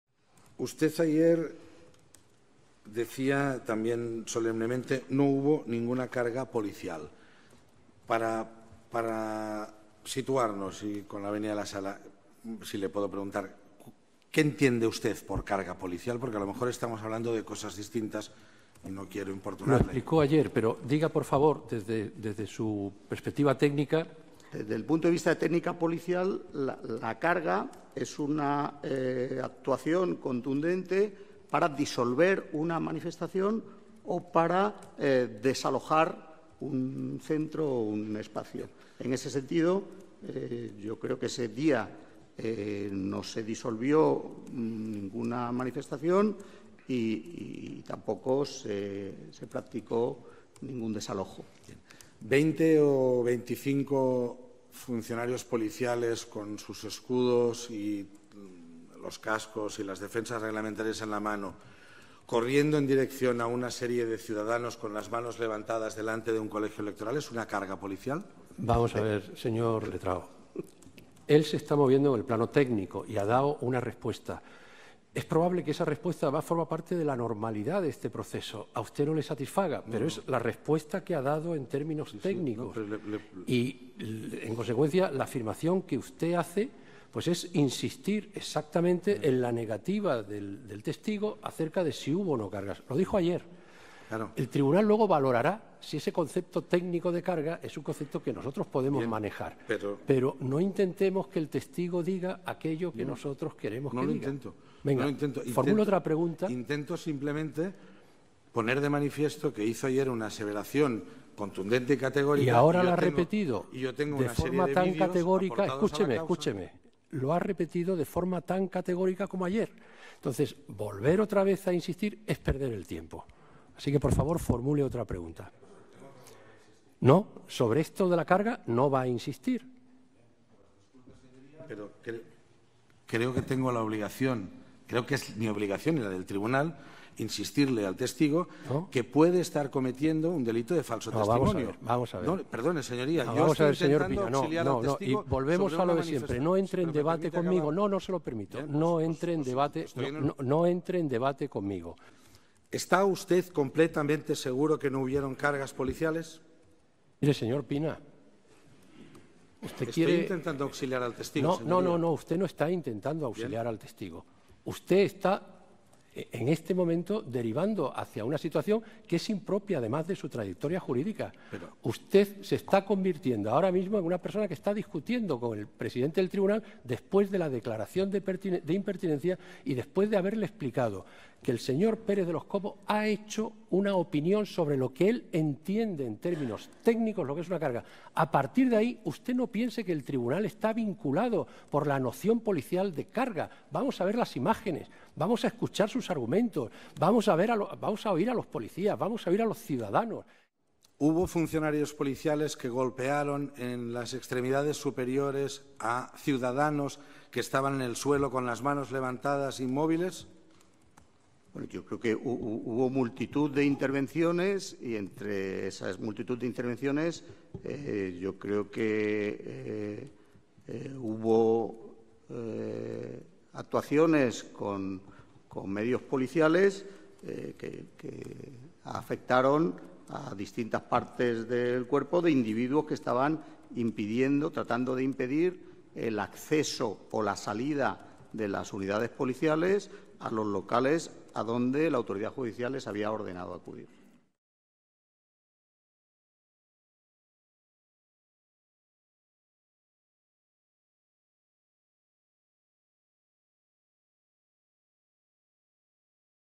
Pérez de los Cobos reitera al abogado Jordi Pina que no hubo cargas policiales el 1-O
El coronel de la Guardia Civil Diego Pérez de los Cobos ha reiterado hoy, en su segunda jornada de declaración en el juicio del procés, que no hubo cargas policiales el 1-O, a preguntas del abogado Jordi Pina.